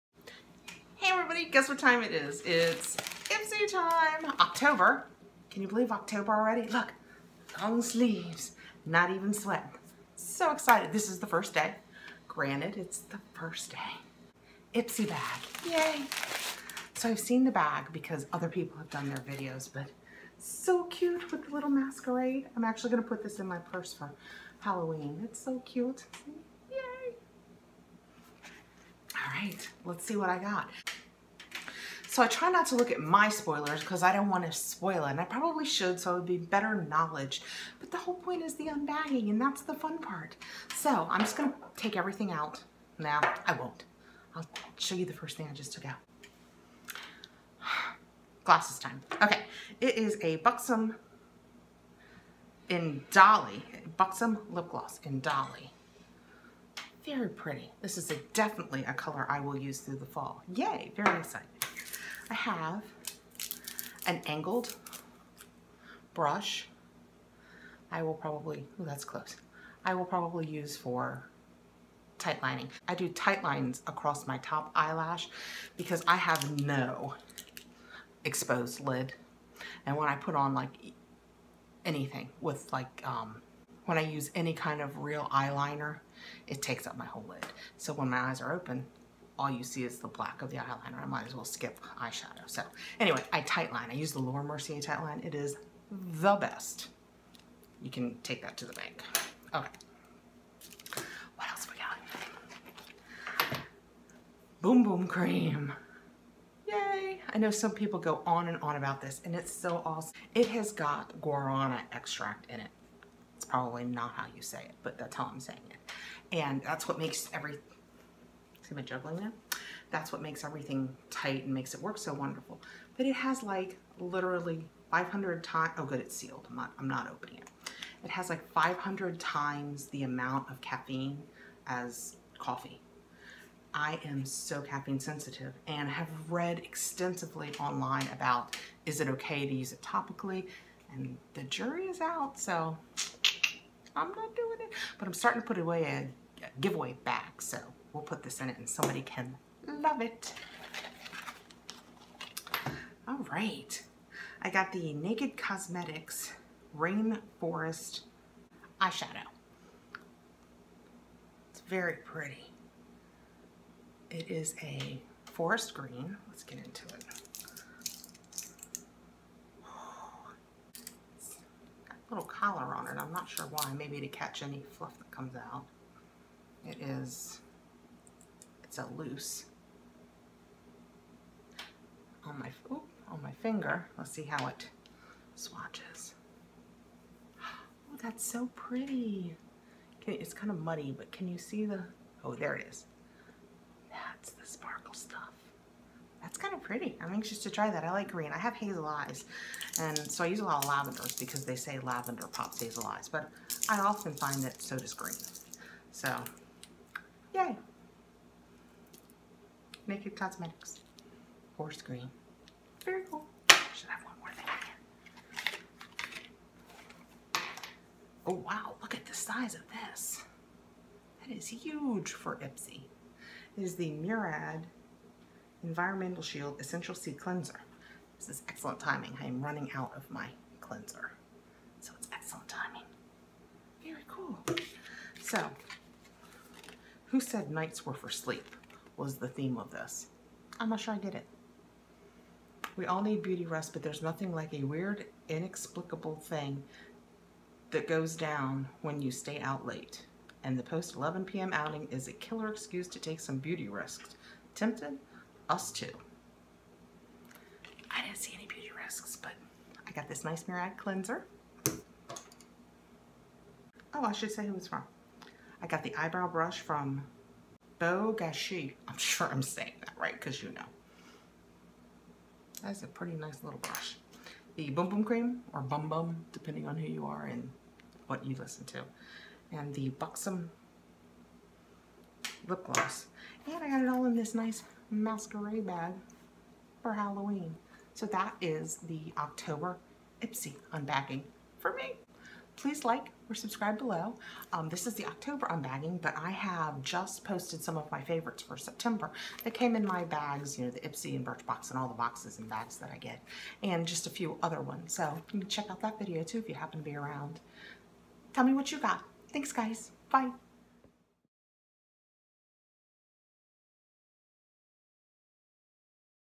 Ipsy Unboxing October 2018
Hi everyone. Thanks for coming by to see my channel.

In today's Unboxing we have:
Buxom Lip Gloss in Dolly
Beau Gachis Eyebrow Brush
Naked Cosmetics Eyeshadow in Rain Forest
Sol de Janeiro Brazilian Bum
Bum Bum Cream
MuradEssential C Cleanser

Link to join Ipsy